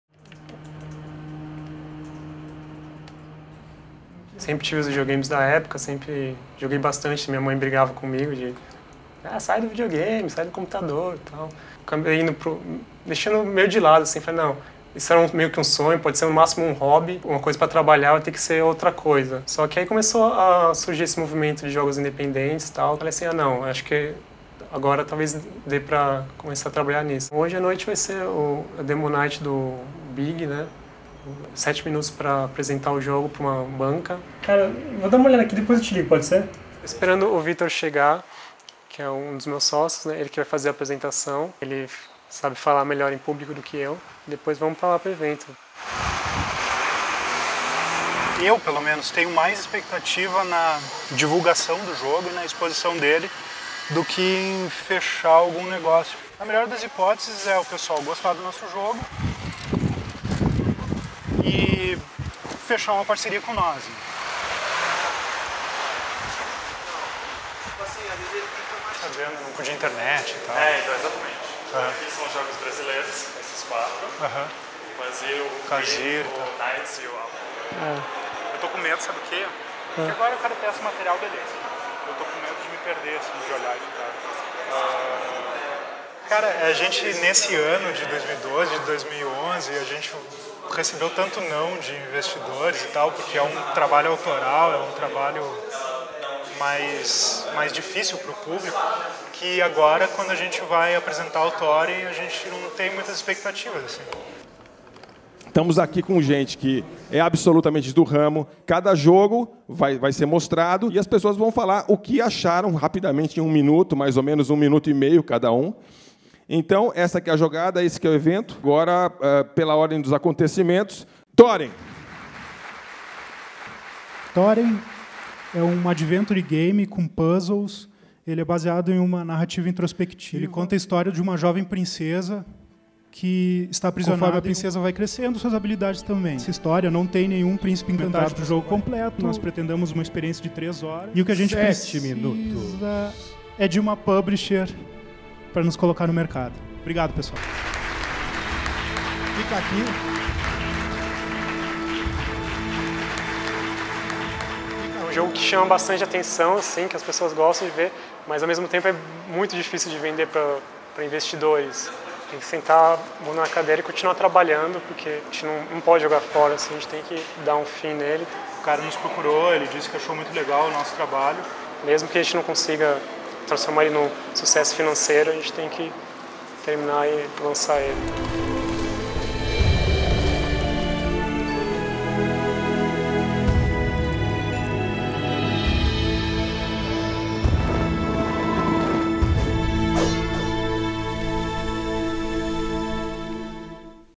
Tentando viver de games no Brasil
Luiz Alvarez e Vitor Leães, produtores do jogo Toren, têm sete minutos para apresentar sua ideia e conquistar o apoio de investidores para levar o título ao mercado.